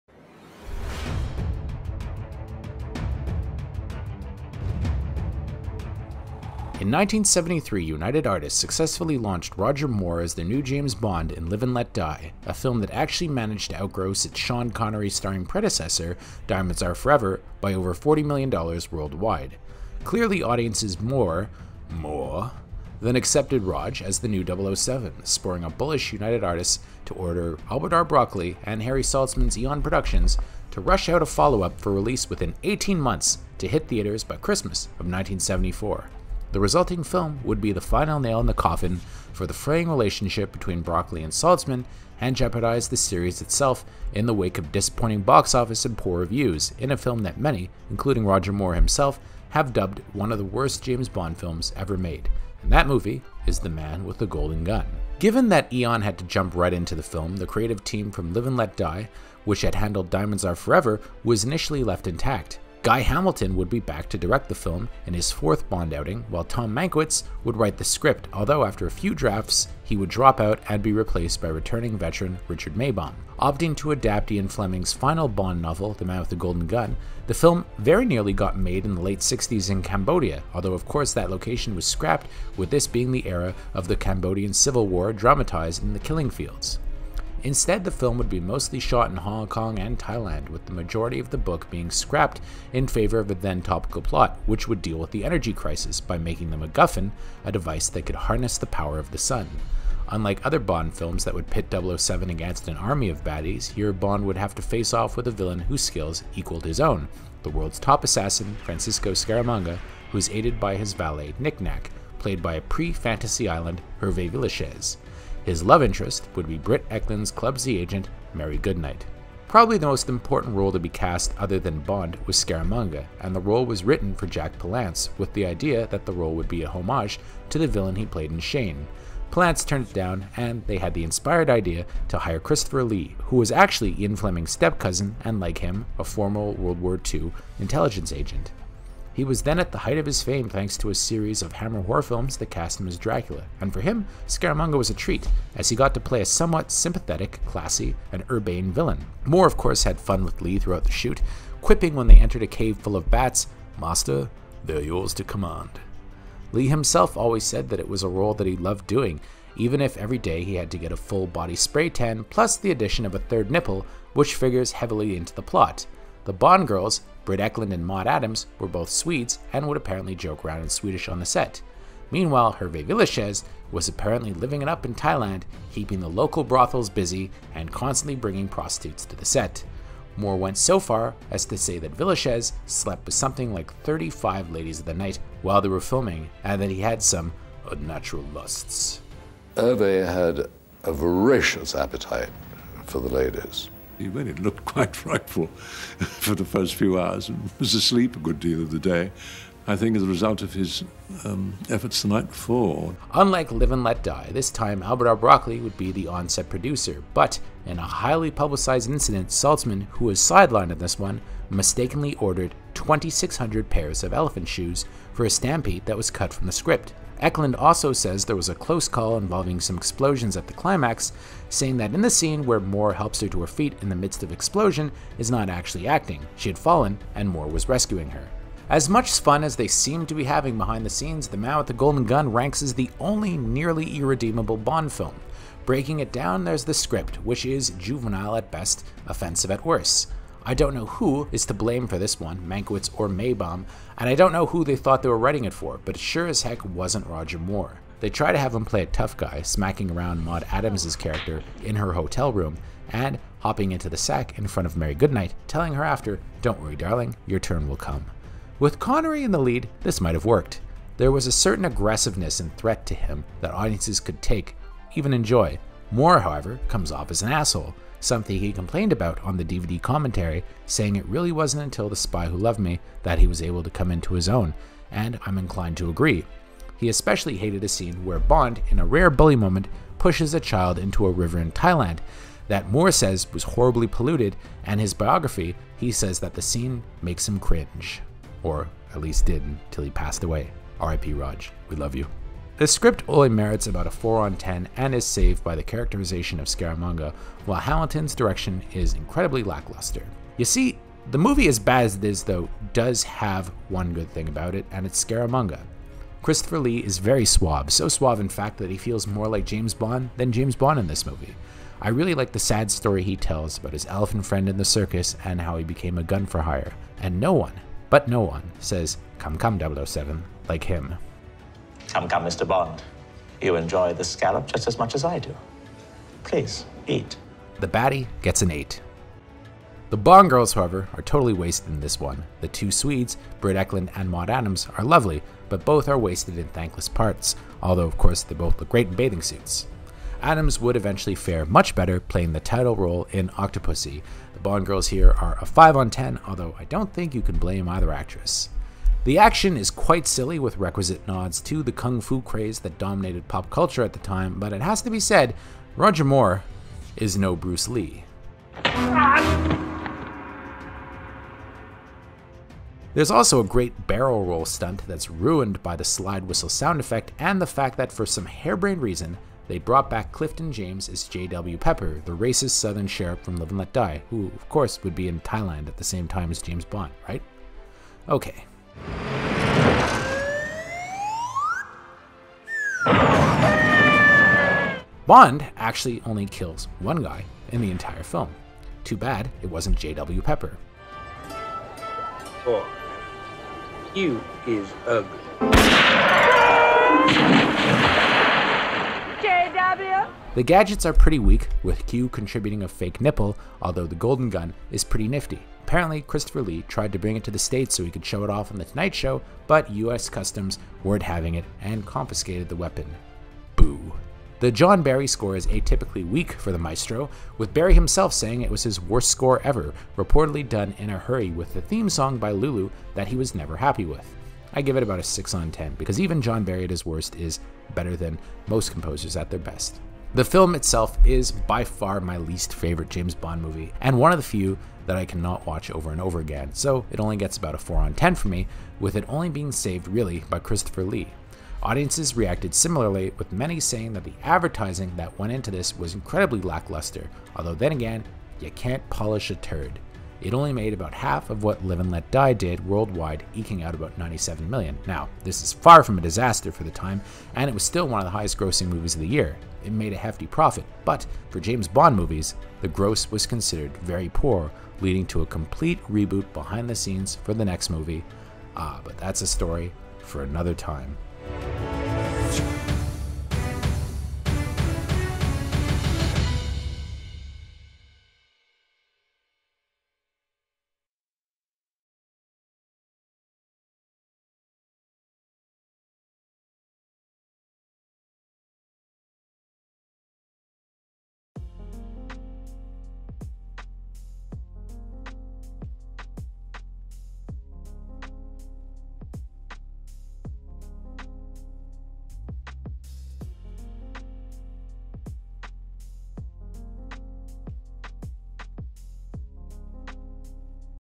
007: THE MAN WITH THE GOLDEN GUN: James Bond Revisited
Welcome back to another episode of "James Bond Revisited"! This time, we're taking a look at Roger Moore's second outing as 007, THE MAN WITH THE GOLDEN GUN, which was actually something of a fiasco for the series, resulting in a soft reboot behind the scenes in the next film, the Bond classic THE SPY WHO LOVED ME. On this one, the relationship between Harry Saltzman and Albert R. Broccoli finally reached its breaking point, and Roger Moore struggled with a poorly conceived Bond adventure, that tried to remold him in the Sean Connery image. It didn't work.

Nevertheless, the film does have some things going for it, including a top-notch villain in Christopher Lee's Scaramanga, two beautiful Swedish Bond girls, Britt Ekland and Maud Adams, and.a strong mano-a-mano showdown between Bond and the baddie. Plus, Hervé Villechaize as Nick Nack (there are some crazy stories related to him in here). Check it out and follow the whole series here: